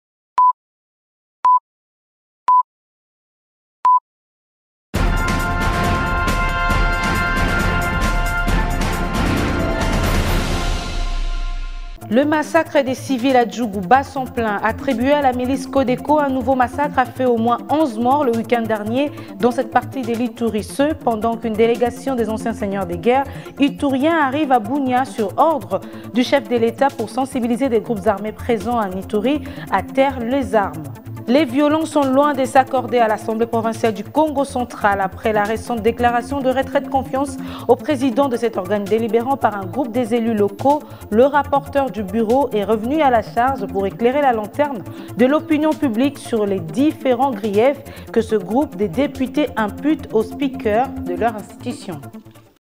Le JT des provinces du 06 Juillet 2020, en bref
1. Ituri : Un nouveau massacre qui fait 11 morts
2. Kongo-Central : Les reproches des Députés à Anatole MATUSILA